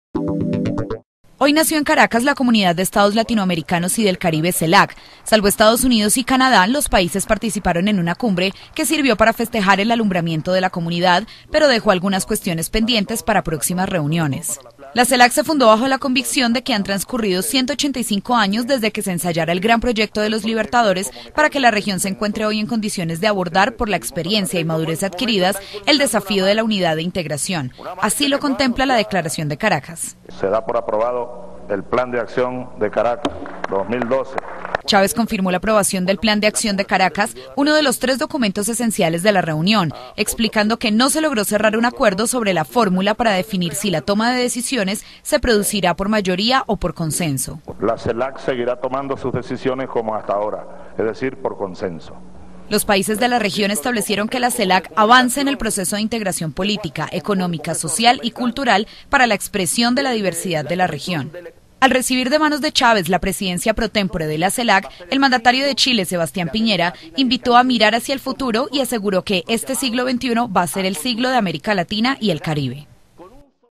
CELAC nace con mucho camino y trabajo por delante
La Cumbre fundacional de la Comunidad de Estados Latinoamericanos y del Caribe concluyó con la aprobación de la Declaración de Caracas y la asunción por parte de Chile de la presidencia.